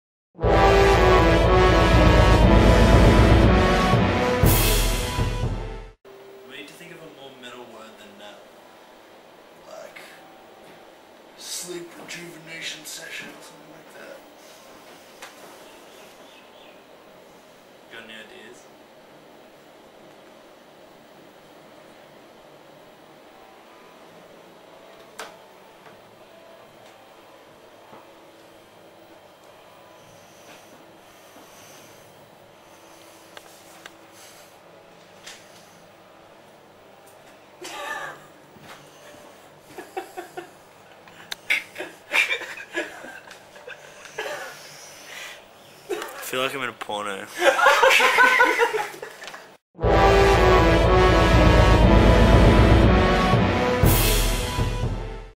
The O'Donnell Chronicles 11
In this mini series we're going to explore the wonderful world that is the mind of rory o donnell. We venture into this world with nothing but an old panasonic MDR-50 dv cam, a 6 pack of melbourne bitter and rory's exceptional videography skills. Enjoy !